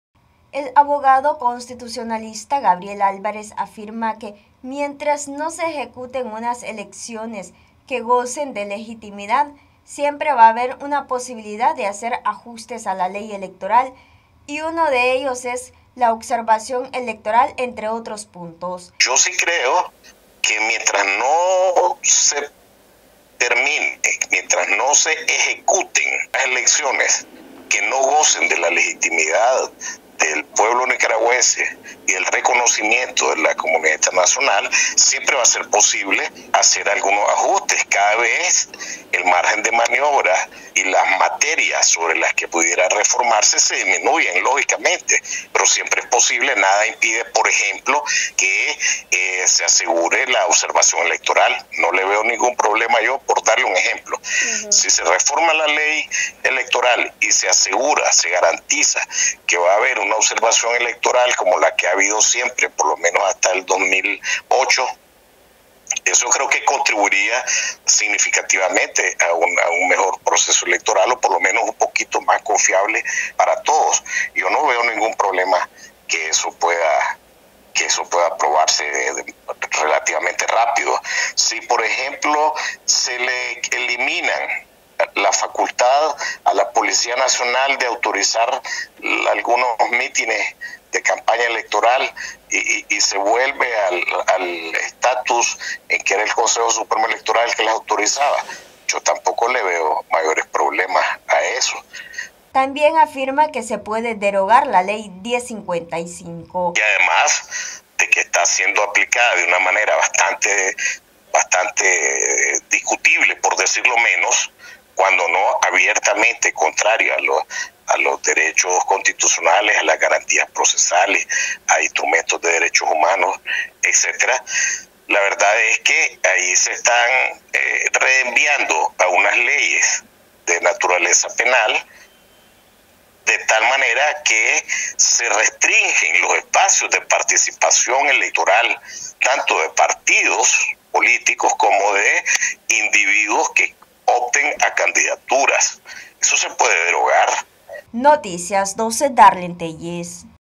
Ley electoral se puede volver a reformar, haciendo ajustes en algunos aspectos
Noticias12Nicaragua Abogado Constitucionalista Gabriel Álvarez afirma que ante demandas de elecciones creíbles aún se puede volver a reformar la ley electoral realizando ajustes en algunos aspectos.